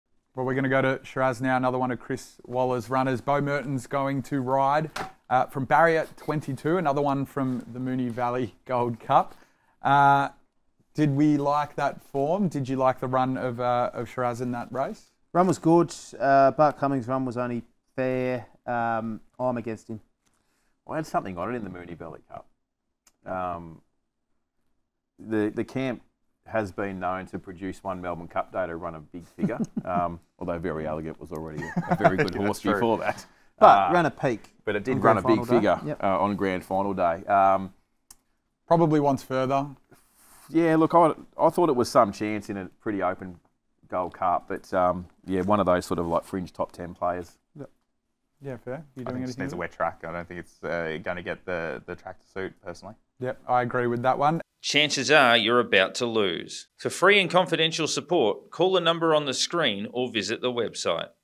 Melbourne Cup | Sheraz
Lay Back with Betfair's Melbourne Cup Special full of this year's Melbourne Cup tips and predictions from a special panel.

Hosted by Karl Kaldawi, he is joined by regular co-hosts Tom Haylock, Shane Ciurleo and Andrew Hawkins.